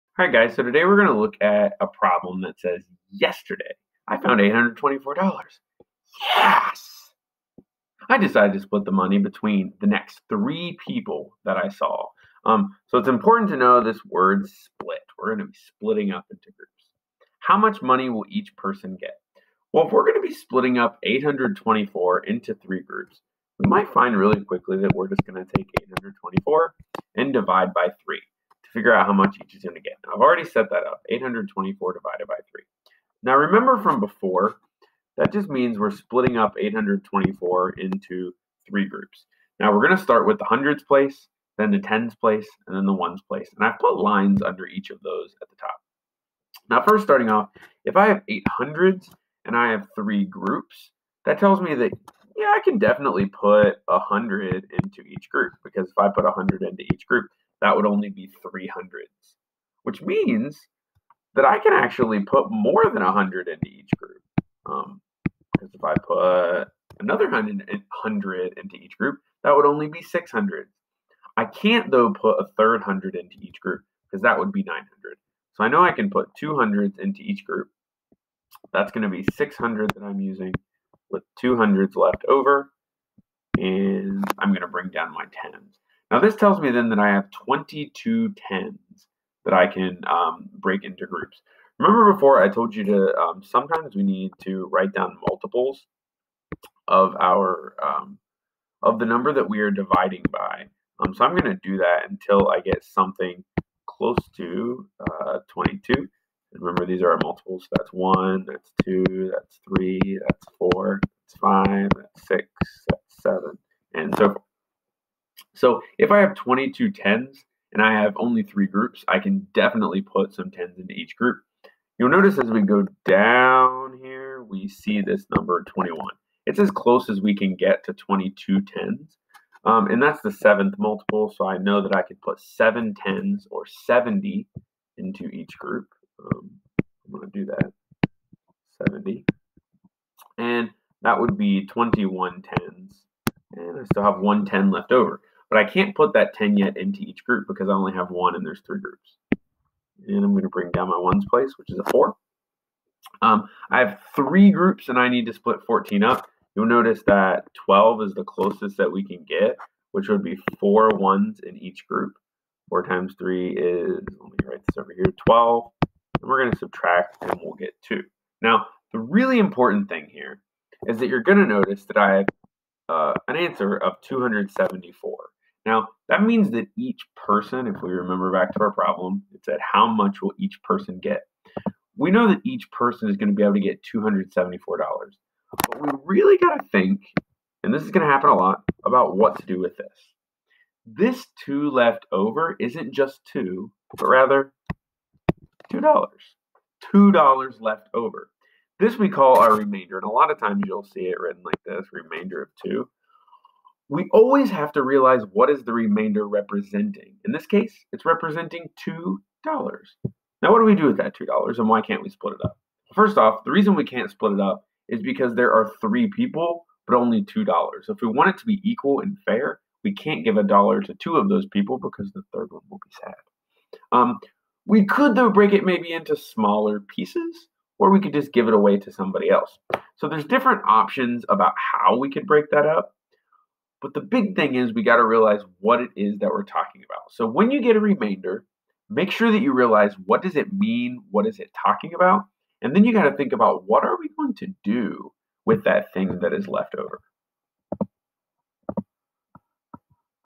Dividing: What Do I Do With a Remainder?
What happens when you get something left over when you divide? This is called a remainder. Watch to find out what this means and how we can use it.